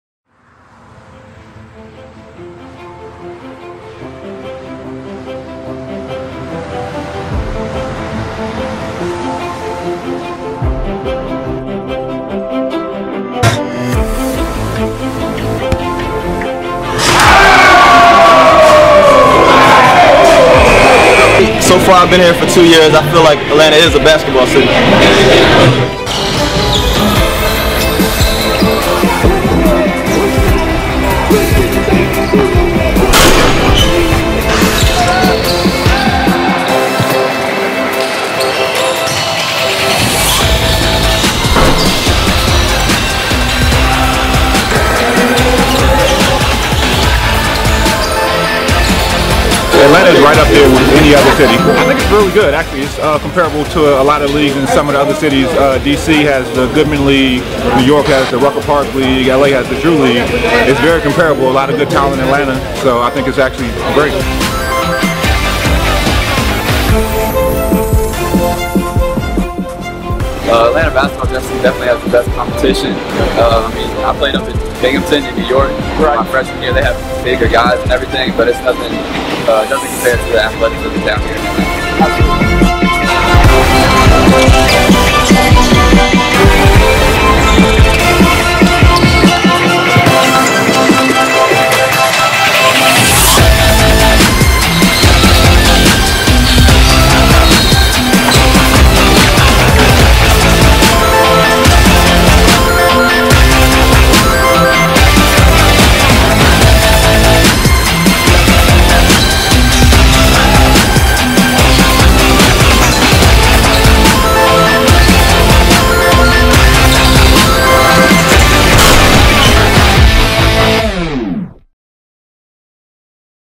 We Ball Y'all
The 2013 Summer Basketball scene in the ATL featured some of the best hoops in the country.  And as the off-season home to professional athletes and entertainers, it wasn't lacking for star power either...